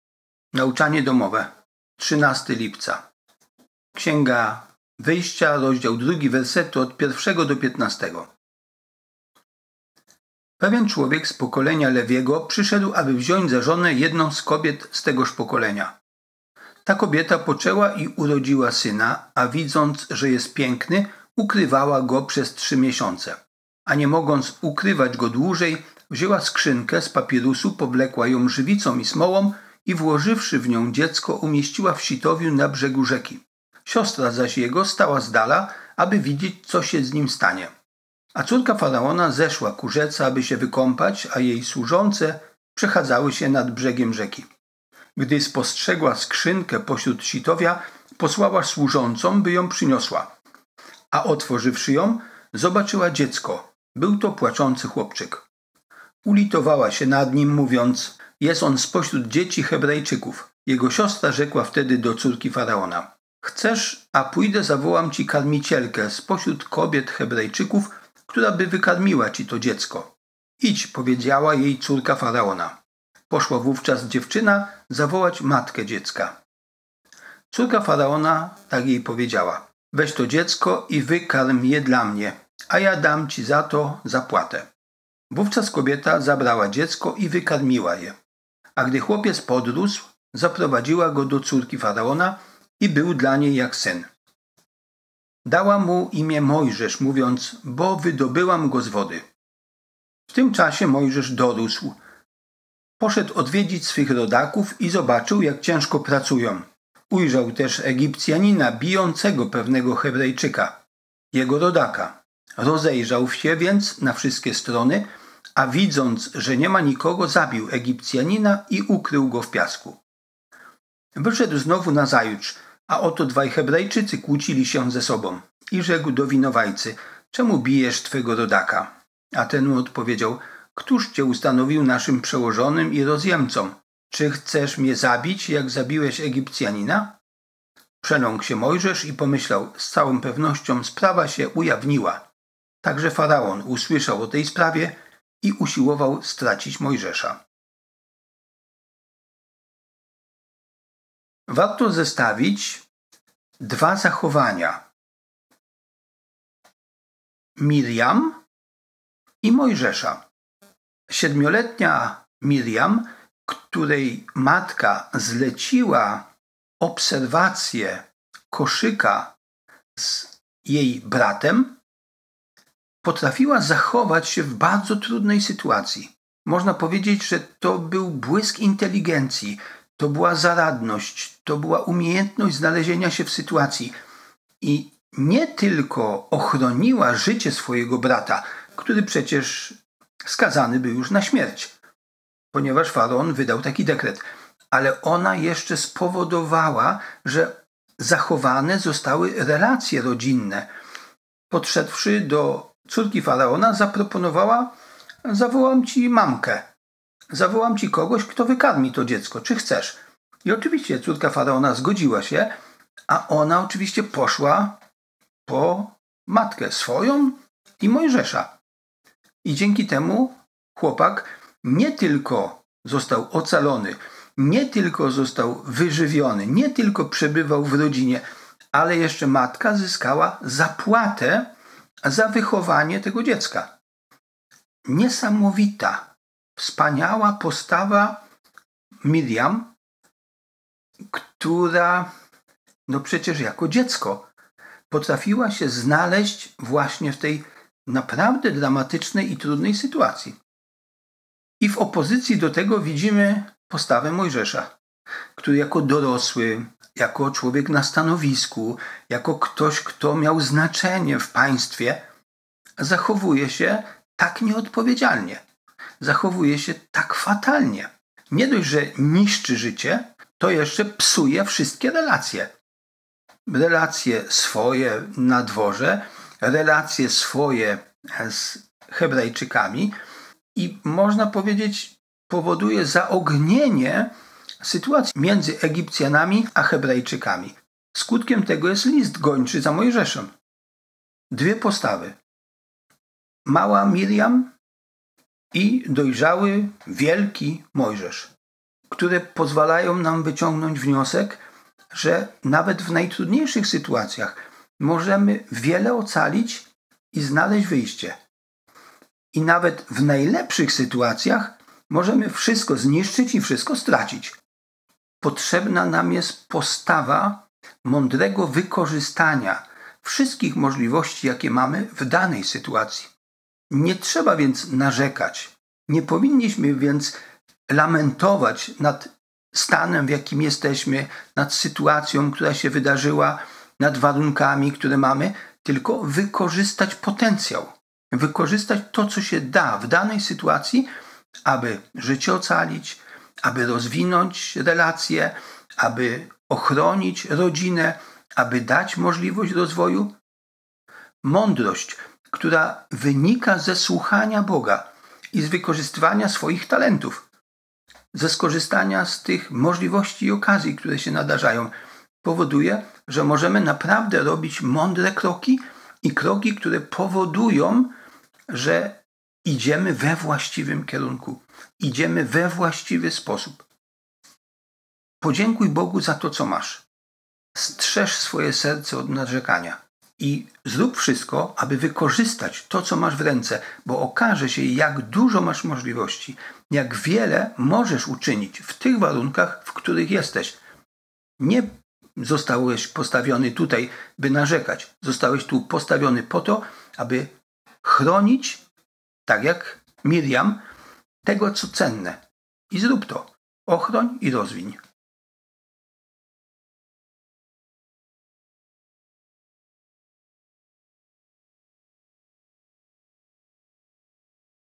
NauczaniaDomowe – L1#101 - Zaradna Marysia - ArturSepioło – 13.07.2021
W najtrudniejszych warunkach można wiele zyskać a z drugiej strony w najlepszych nawet warunkach można wszystko stracić.

Informacje ogólne:
Prowadzący:
Kamera:
Grafika kanału: Robert Mierzwiński